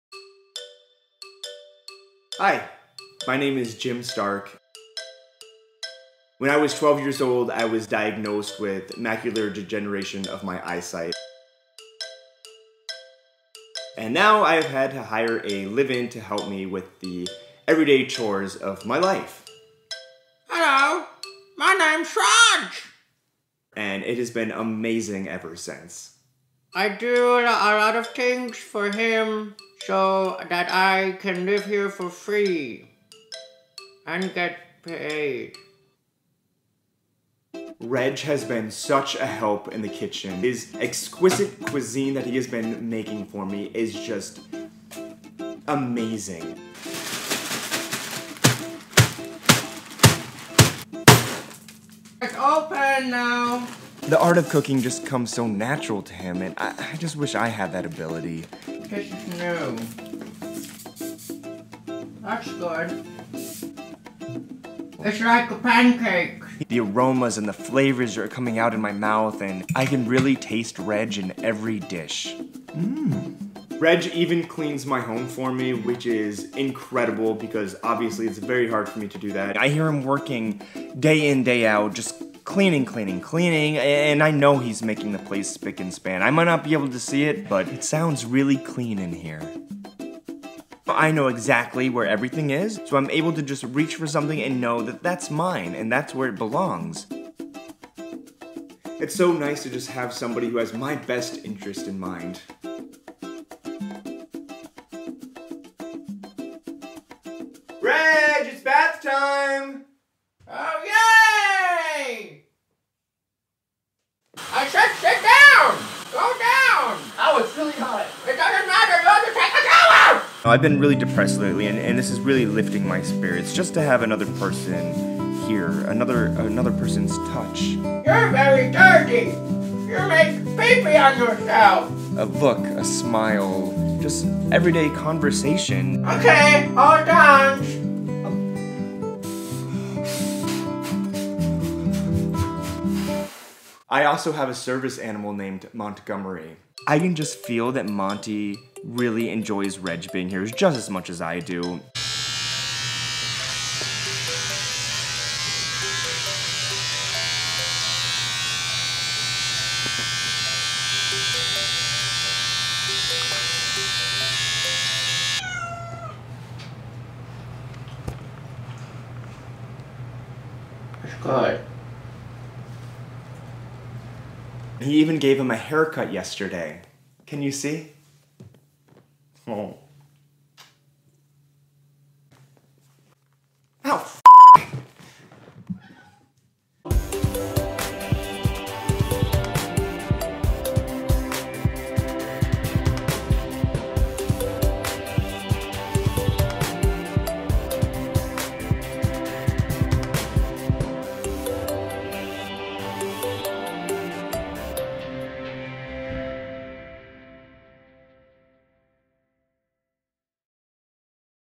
Weird Documentary Show
Jim Stark is a trust fund man who finds himself battling with Macular Degeneration (vision loss). He enlists the help of Reg, a strange guy from an unknown land. We follow them as they embark embark on their new lives together.

Documentary series starring Jim Stark and Reg. Reality TV in the vein of the E! Network, TLC shows, Bravo, HBO.

Comedy, improv, sketch, funny, weird.